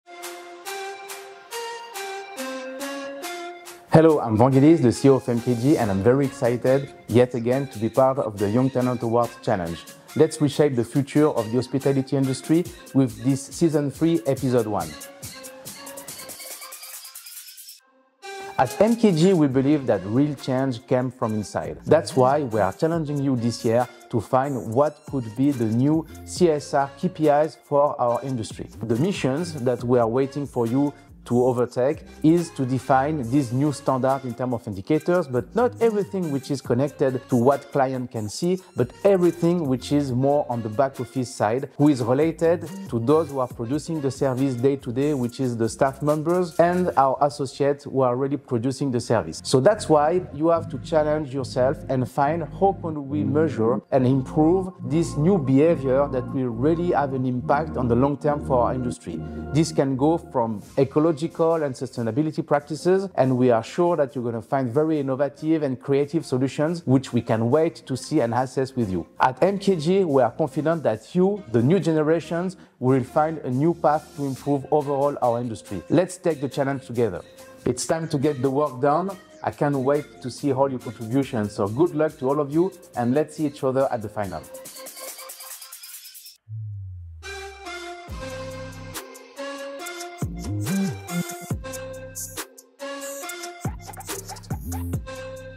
Young Talent Awards 2024 Season 3 Episode 1 x MKG Consulting
🤝 Through the eyes of MKG Consulting, your mission is to explore the Corporate Social Responsibility as you’re invited to define new indicators for the hospitality industry, with peak attention to practical implementations, sustainability and social awareness.